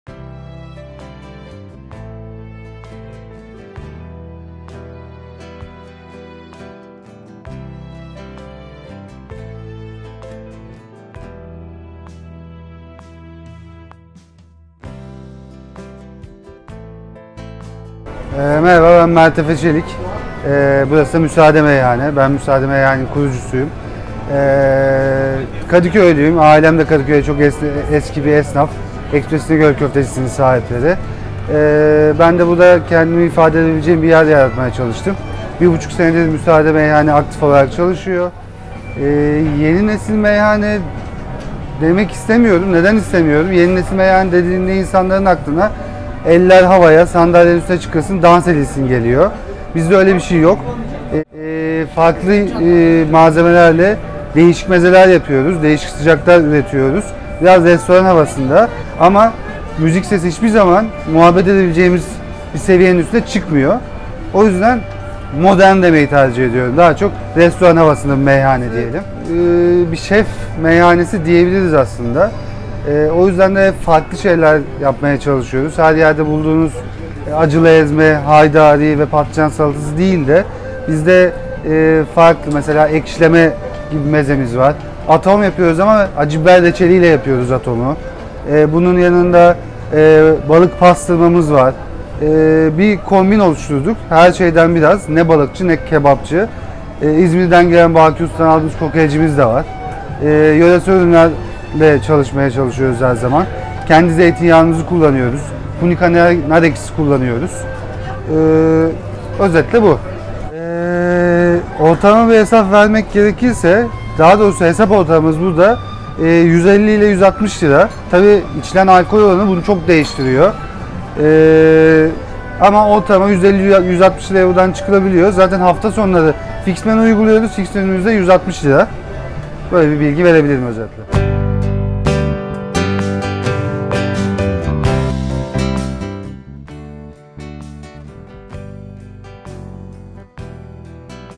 Müsaade Meyhane
Müsaade Meyhane Kadıköy'de yer alan, kendi deyişleriyle biraz restoran havasında bir meyhane. Çünkü yaptıkları farklı yemeklerle ön planda bir meyhane. Diğer meyhanelerdeki alışa gelmiş mezelerin ve sıcakların dışında bir şeyler yaratmaya çalışıyorlar ve bundan dolayıda aslında kendilerine şef meyhane söyleminde de bulunabiliyorlar.
Haftasonu  fix menüsü olan meyhaneye rezervasyon yaptırmadan gitmemenizi tavsiye ederiz.

🌍 Kadıköy
⏰ 14:00- 01:00
💸 300₺ (iki kişi ortalama)
🏠 Meyhane
🍽 Meze çeşitlerif